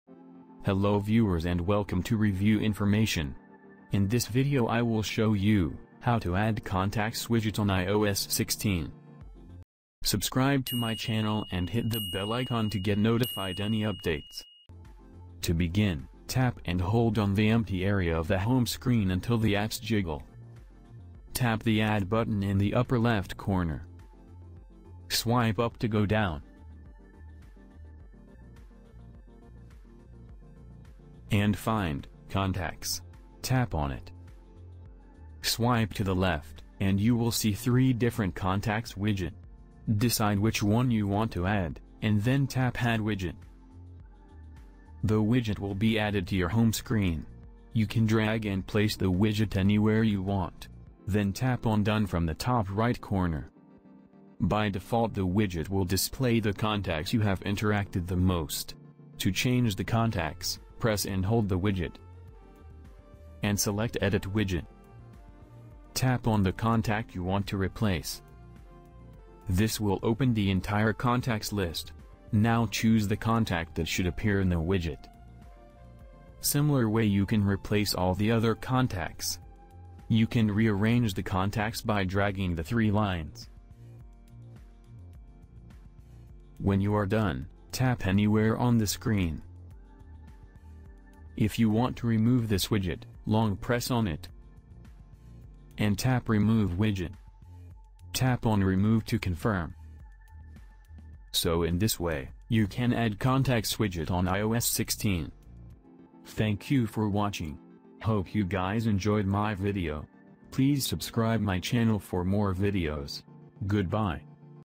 How to Add Contacts Widget on iOS 16
In this video I will show you, How to Add Contacts Widget on iOS 16.

To begin, tap and hold on the empty area of the home screen until the apps jiggle. Tap the Add button in the upper-left corner. Swipe up to go down, and find, contacts. Tap on it. Swipe to the left, and you will see three different contacts widget. Decide which one you want to add, and then tap add widget. The widget will be added to your home screen.

Tap on done from the top right corner. You can drag and place the widget anywhere you want. Then tap on done from the top right corner. By default the widget will display the contacts you have interacted the most. To change the contacts, press and hold the widget, and select edit widget. Tap on the contact you want to replace. This will open the entire contacts list. Now choose the contact that should appear in the widget.

Similar way you can replace all the other contacts. You can rearrange the contacts by dragging the three lines. When you are done, tap anywhere on the screen. If you want to remove this widget, long press on it, and tap remove widget. Tap on remove to confirm.